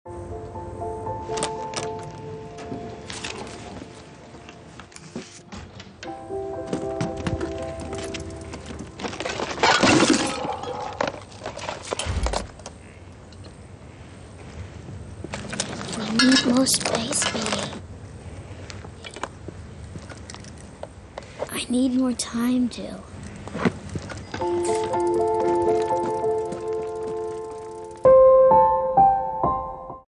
ŠKODA Octavia TV Commercial from 2010 - The Great Escape
TV advert for the ŠKODA Octavia, aired in 2010 - The Great Escape. Find the full range of ŠKODA vehicles in New Zealand at Miles Continental